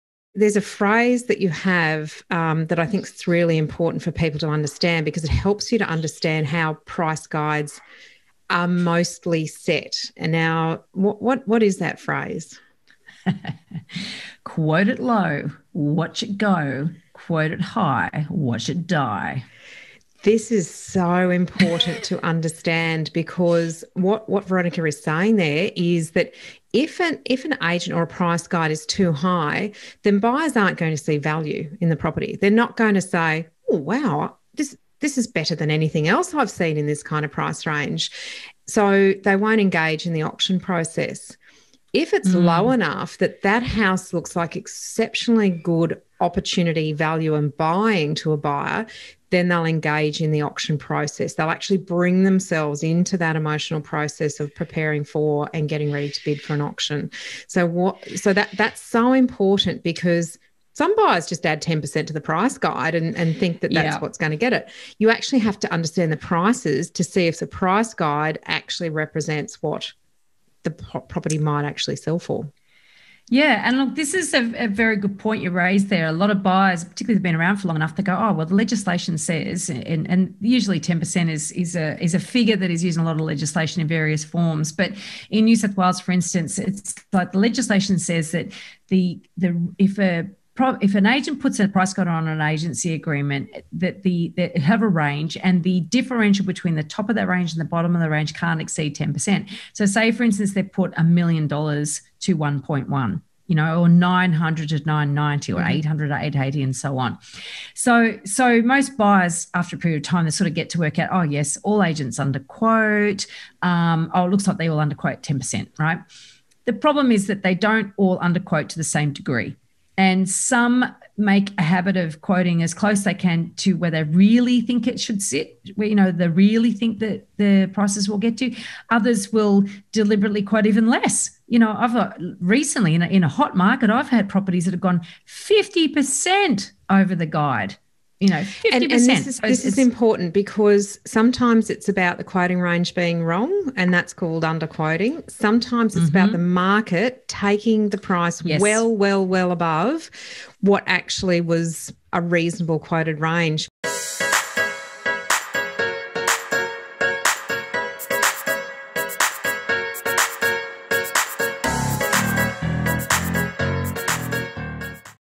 A Phrase That Describes How Most Price Guides Are Set
If you’re planning to put your property in for an auction, you might be struggling with how to price the property correctly.

In this short clip from our episode on auctions (coming out on Wednesday), we share a phrase that will help you understand how price guides are set.

We unpack a lot of insight in the full episode so don’t forget to tune in on Wednesday!